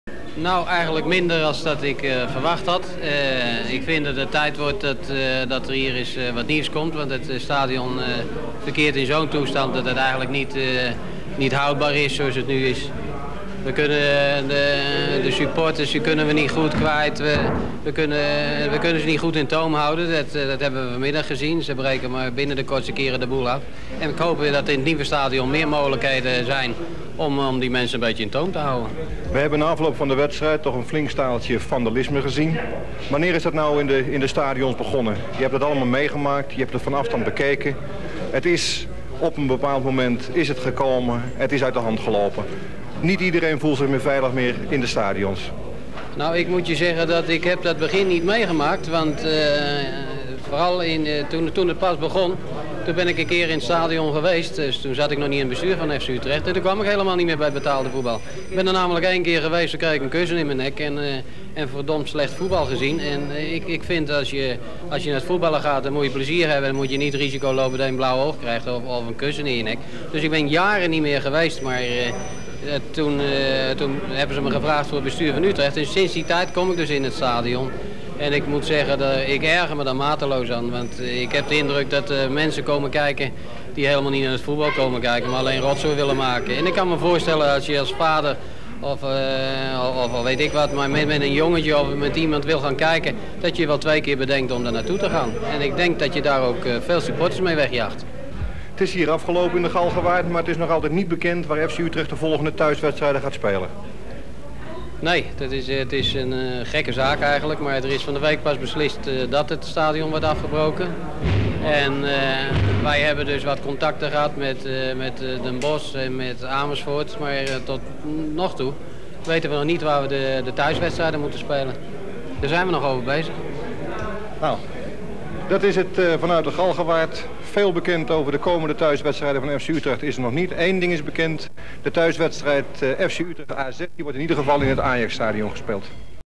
Over het slopen van Galgenwaard 1981
Hans Kraay sr & Tonnie van der Linden over de
 sloopwerkzaamheden door de fans.
Datum van uitzending:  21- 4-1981
Bron: Phlips n1700